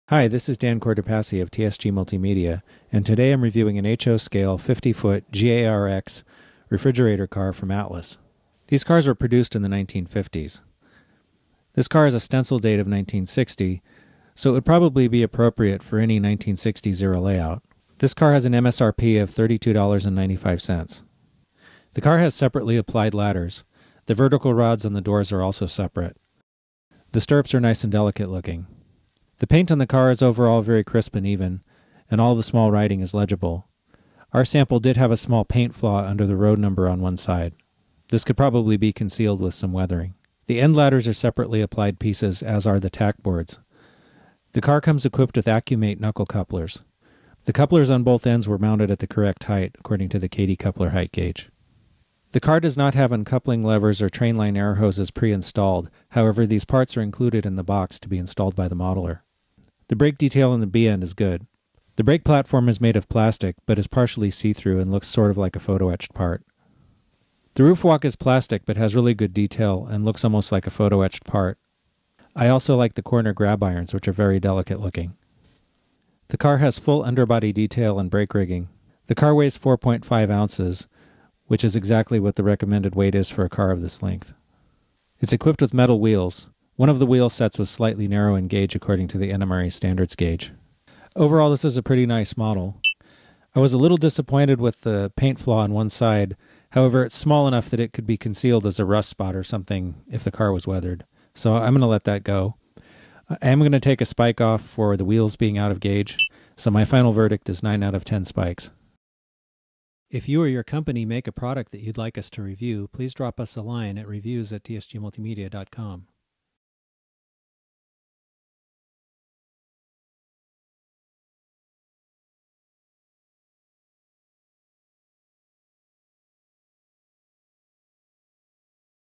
HO Scale Refrigerator Car UP Reefer Atlas Product Review
Refrigerator cars used to be a very common sight on prototype railroads. Not so much now, except on model layouts. This HO scale reefer from Atlas captures the look of one of these reefers quite well. See how it rates on our Golden Spike Rating System™ in this review.